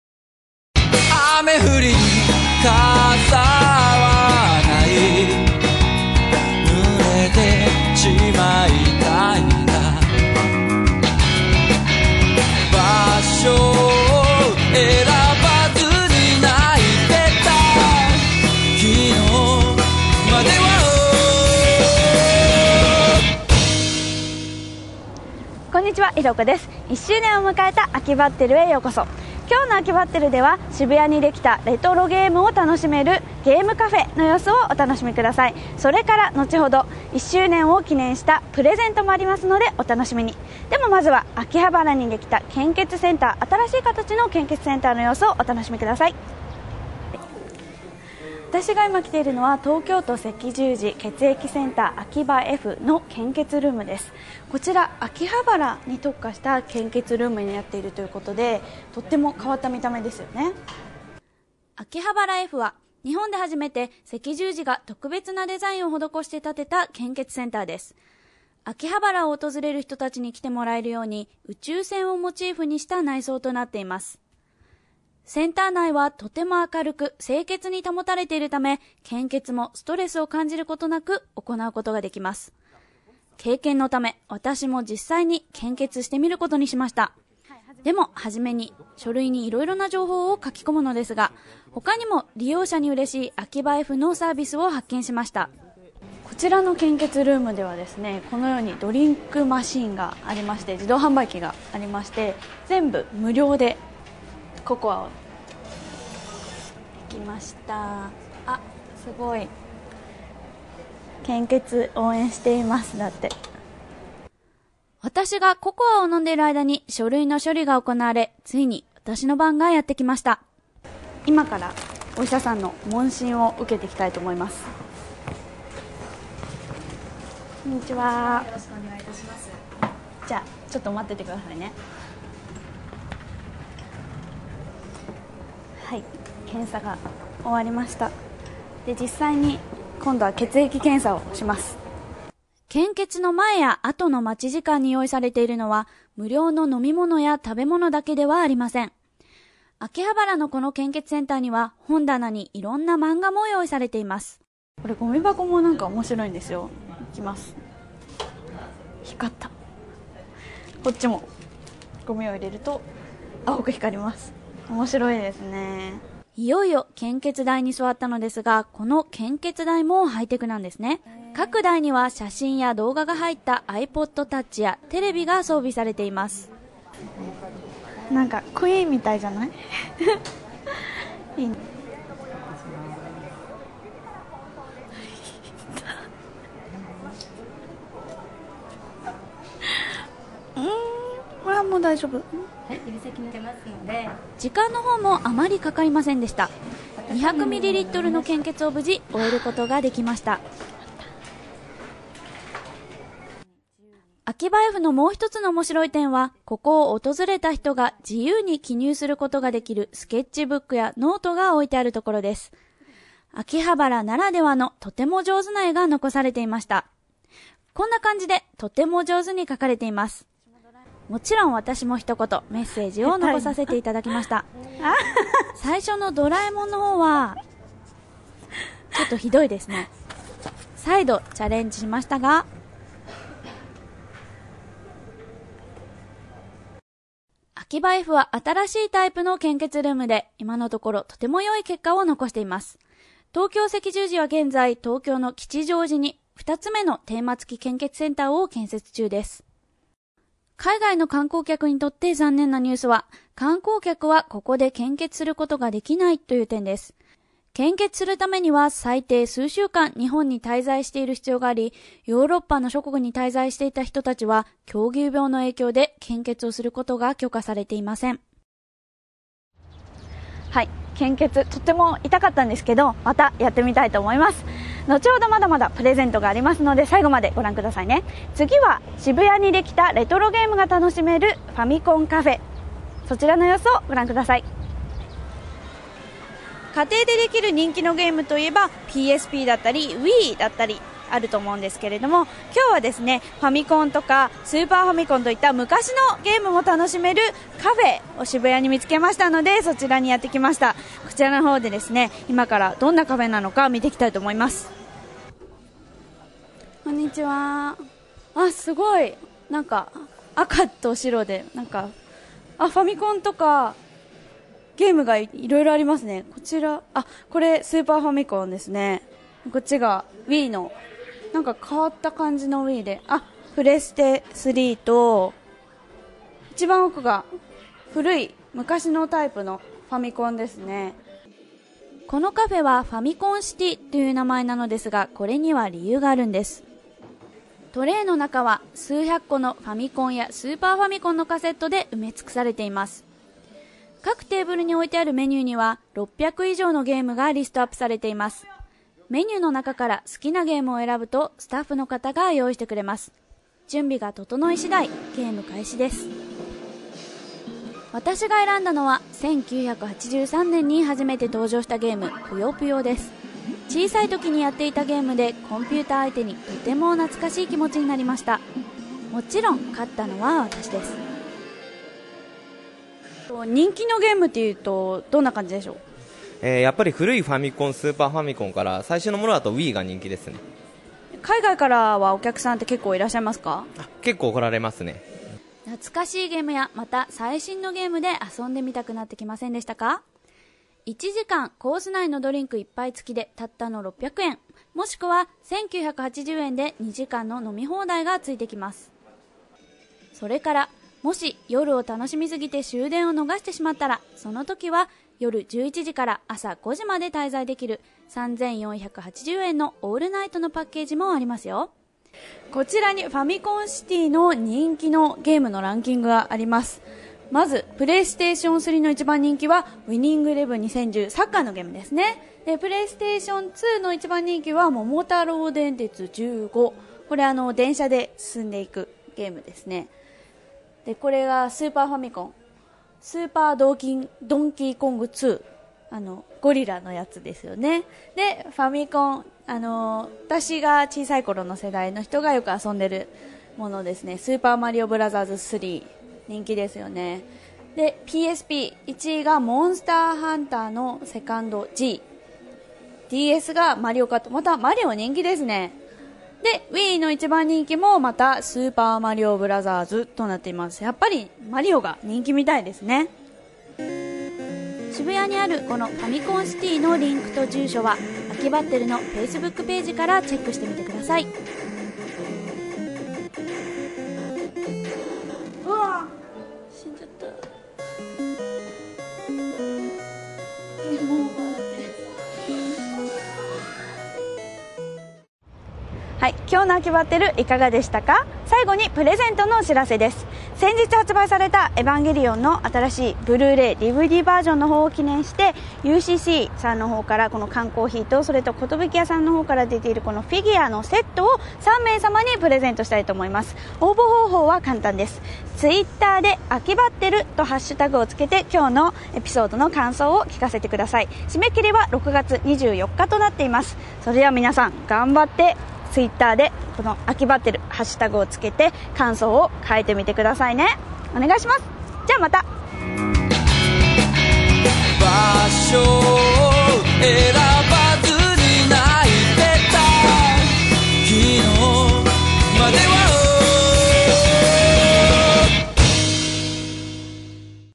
Akibatteru 14: Retro gaming at Famicon City and Akihabara's spaceship-style blood bank
In this episode of Akibatteru, Hiroko visits a new gaming cafe in Shibuya that offers modern and retro gaming and discovers if she still has what it takes to beat the Super Famicon. She also checks out a spaceship-styled blood bank in Akihabara, and ends up getting relieved of 200ml of her own blood. Plus, to celebrate our first anniversary, we have a competition.